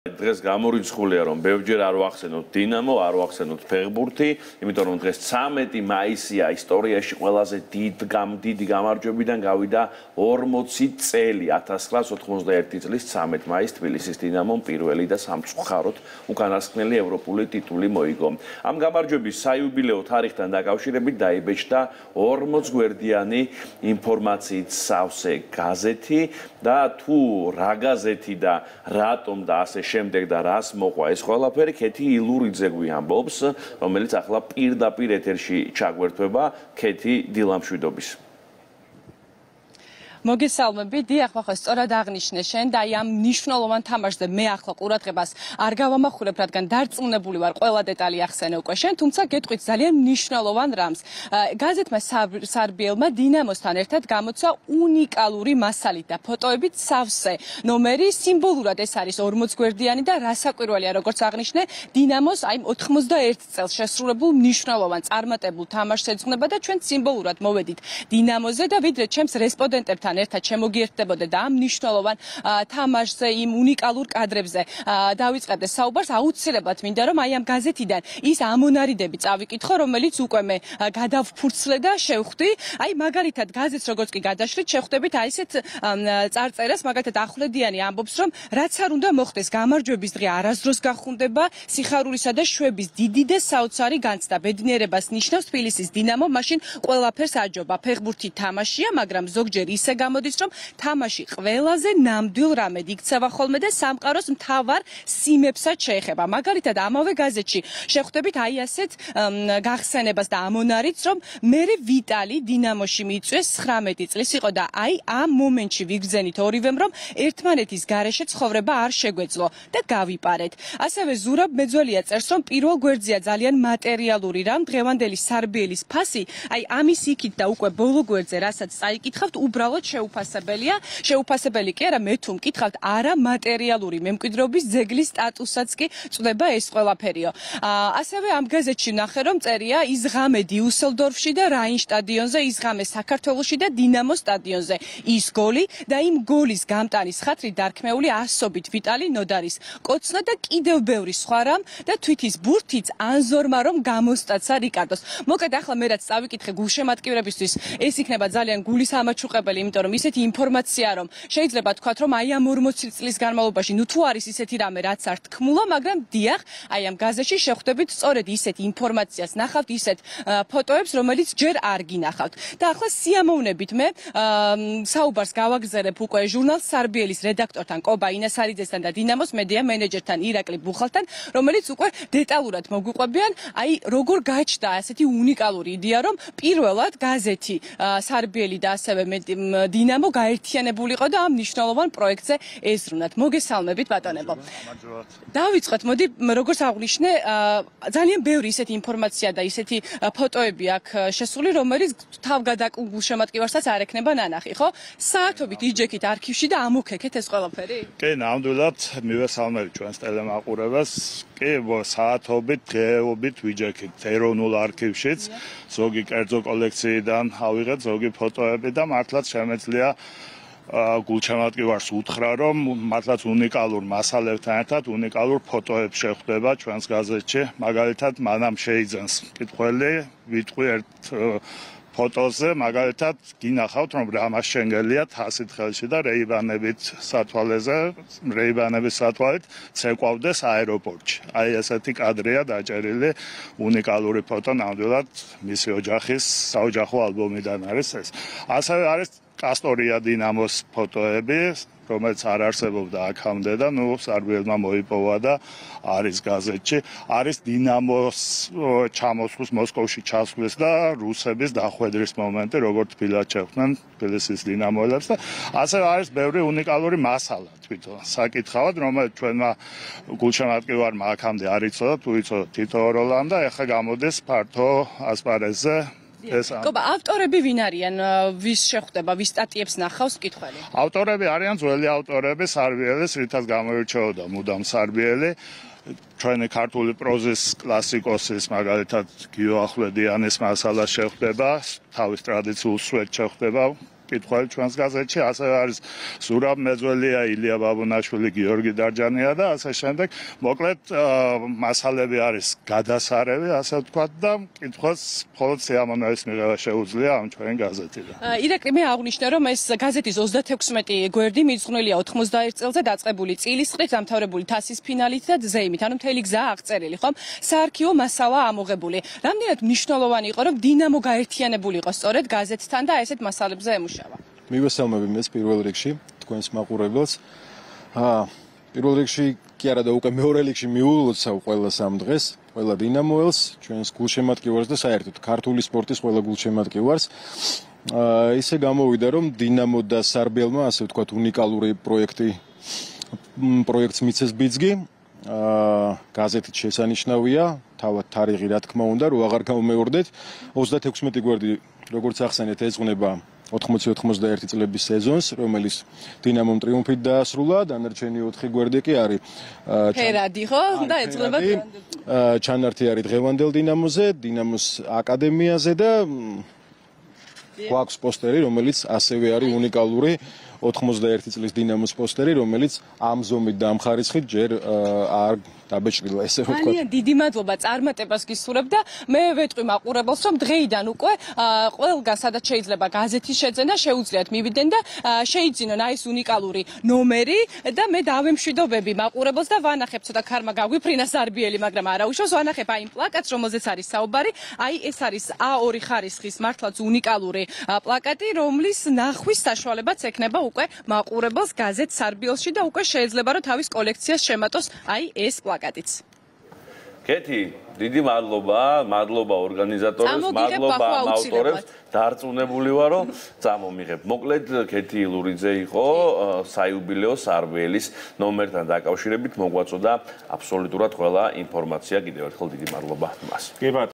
გაზეთ სარბიელის სპეციალური გამოშვება დინამოს გამარჯვების საიუბილეო თარიღის აღსანიშნავად
🏆 40 წელი ქართული ფეხბურთის ისტორიაში ყველაზე დიდი გამაარჯვებიდან